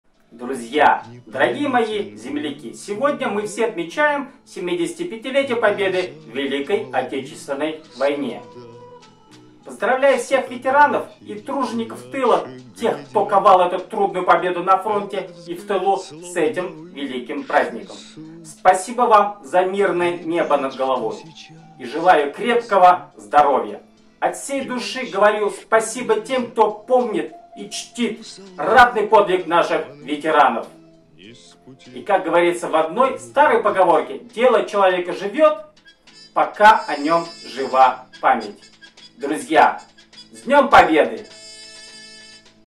С ДНЕМ ПОБЕДЫ
Друзья, дорогие мои земляки👫. Сегодня, мы все отмечаем 75 летие Победы в Великой Отечественной войне. Поздравляю всех ветеранов и тружеников тыла, тех кто ковал эту трудную победу на фронте и в тылу, с этим великим праздником🌟. Спасибо всем вам, за мирное небо над головой🌤, и желаю крепкого здоровья. От всей души говорю спасибо тем, кто помнит и чтит, ратный подвиг наших ветеранов🎖. Как говорится в одной старой поговорке, дело человека живёт пока о нем жива память🔥. Друзья с Днём Победы!💥💥💥